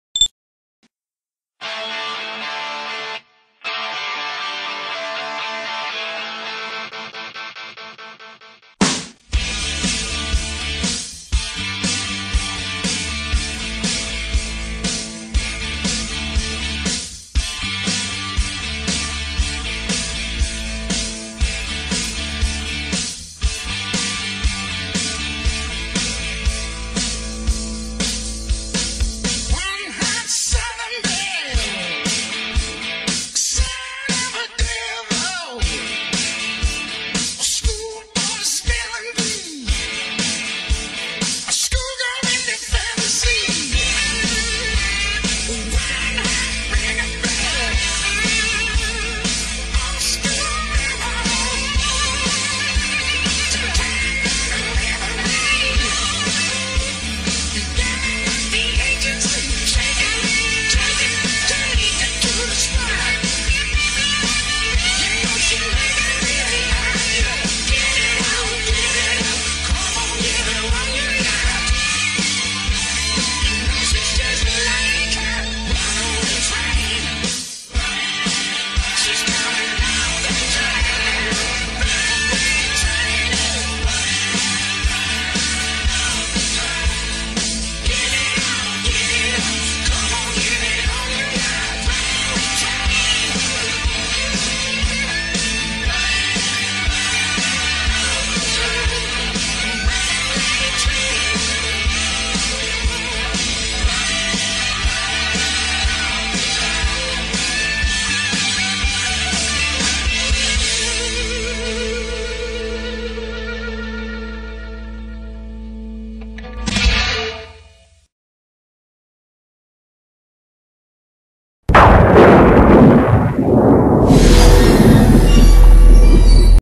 Run Right Off The Track
Some race clips I got off of RuTube. Have no frigg'n idea where or when the race was, but being a driver, I'd love to give it a try! Edited AC/DC in the background.